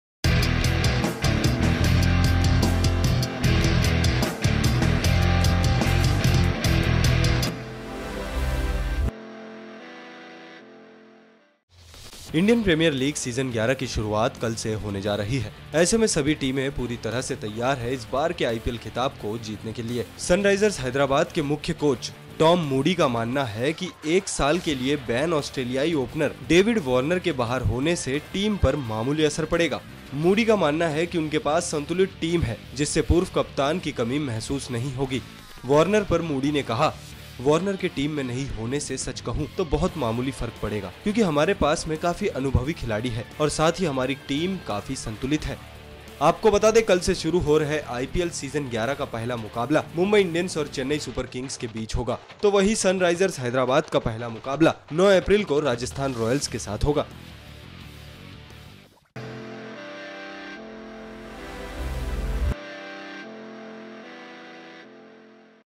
Tom Moody ने कहा की Warner की कमी नहीं खलेगी Team को, जाने आखिर क्यों
Sunrisers Hyderabad (SRH) had to replace their skipper David Warner with New Zealand captain Kane Williamson after BCCI barred the Australian from the 2018 Indian Premier League (IPL), following the one-year ban imposed by Cricket Australia.